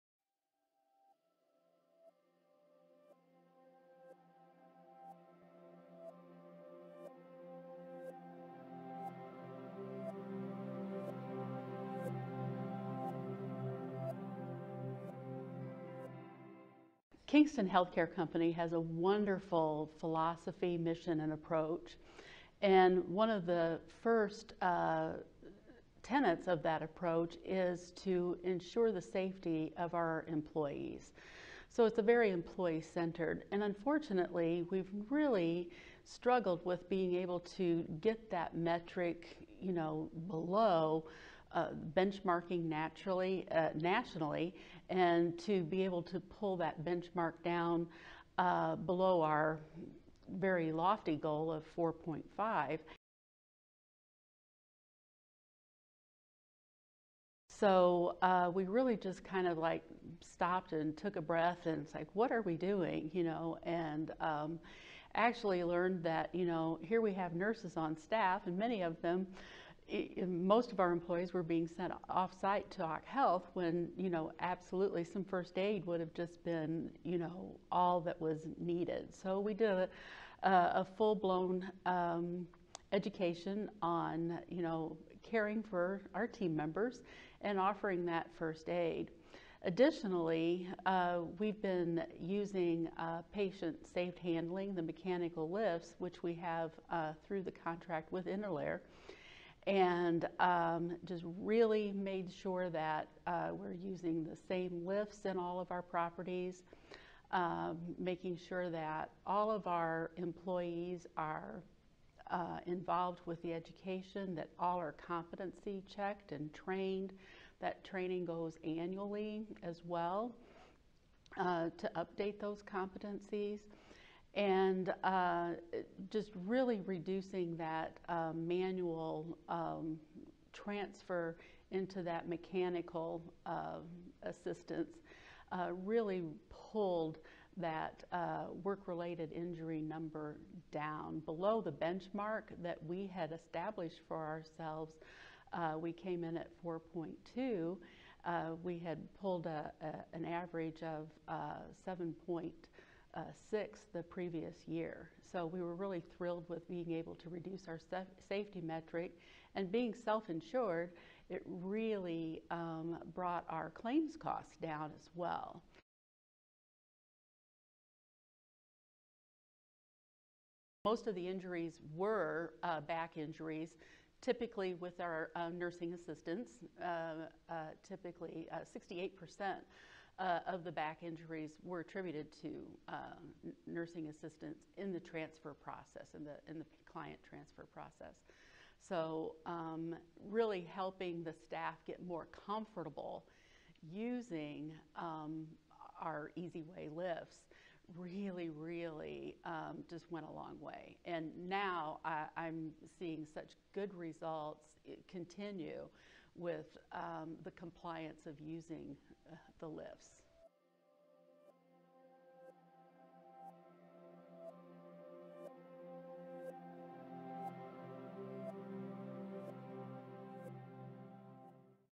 Kingston HealthCare Company | Improved Employee Safety & Reduced Workers’ Comp Expenses
By providing first aid onsite to injured workers when appropriate, and installing standardized patient lifting equipment to reduce back strain injuries, Kingston HealthCare Company reduced its OSHA recordable incidence rate from 7.6 to 4.2 and workers’ compensation costs by 24.7% in 2016. This project resulted in an Intalere Healthcare Achievement Award.